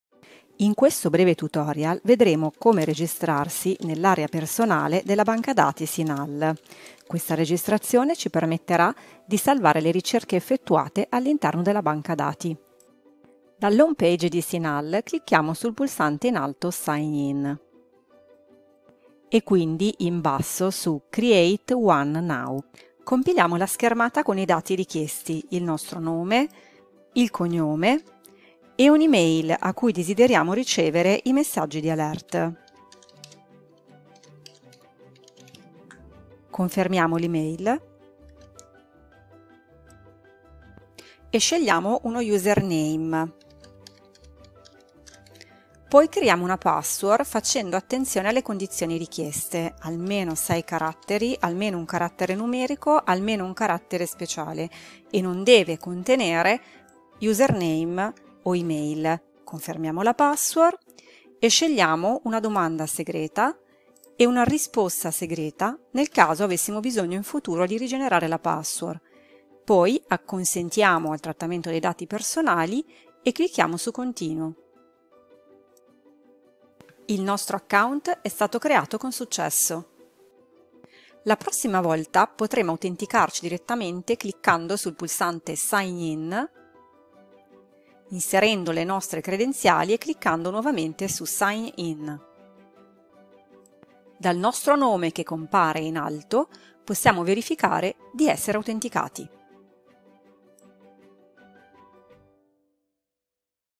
Cinahl: registrazione
Come registrarsi nell'area personale della banca dati Cinahl.